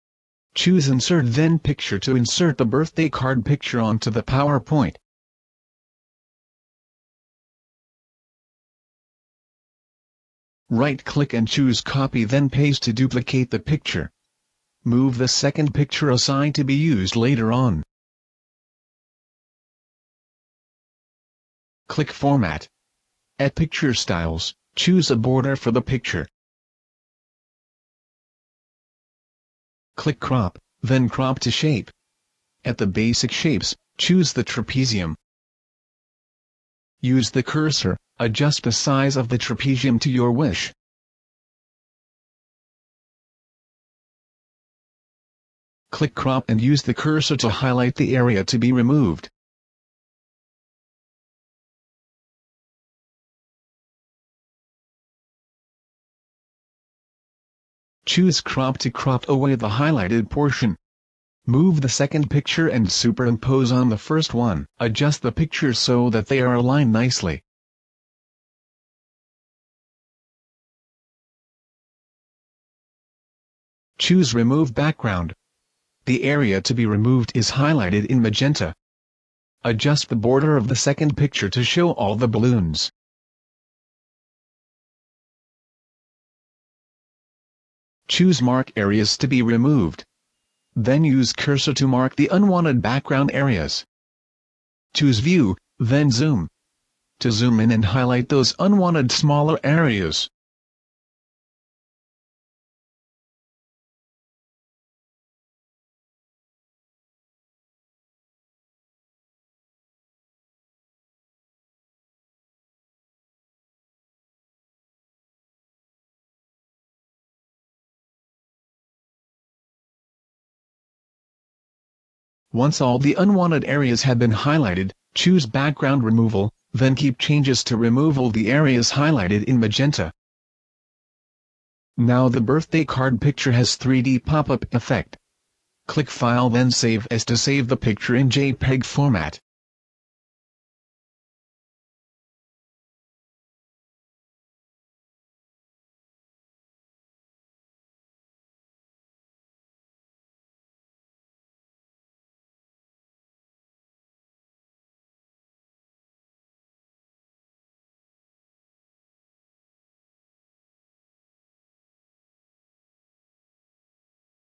How To Make Birthday Greeting Card Picture to have 3D Popup Effect using Powerpoint
This video shows how to make a Birthday card picture to have 3D popup effect using  Powerpoint.